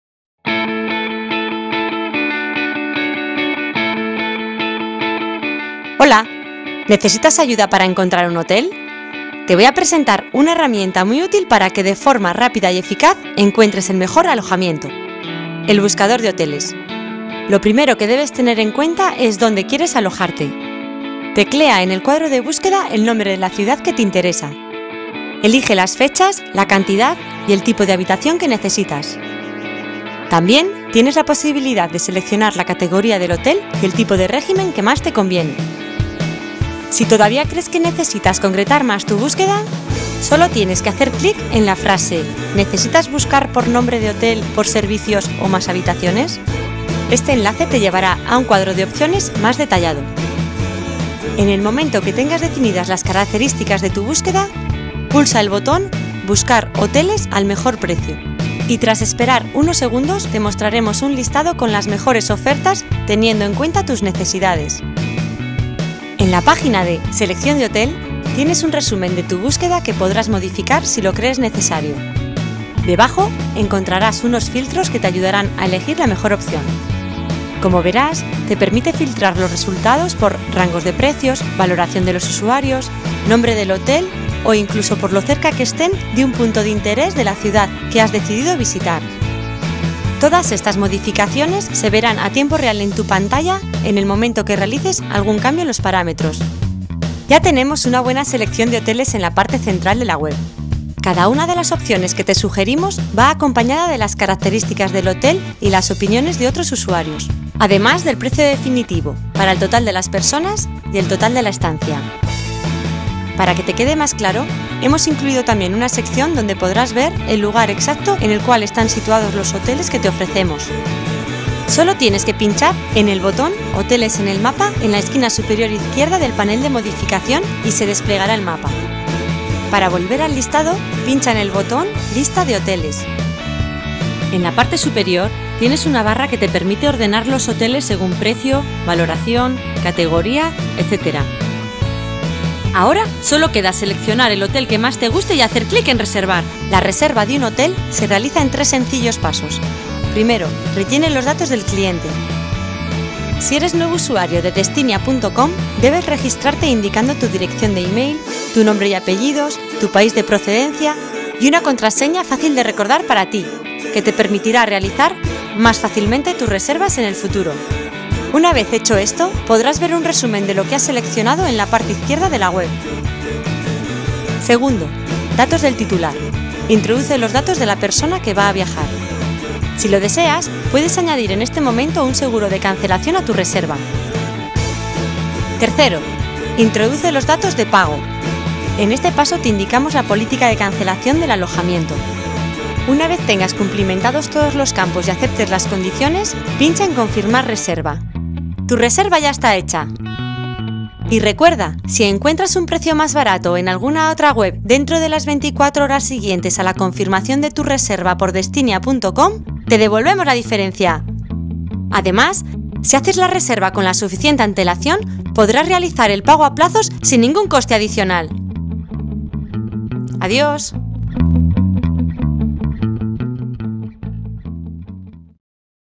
Tutorial Resevas de Hoteles | Destinia
Videotutorial de reservas de Hoteles con Destinia.com
Aprende cómo en encontrar la mejor oferta para tus reservas de Hoteles, y confirmar tu reserva de una manera fácil.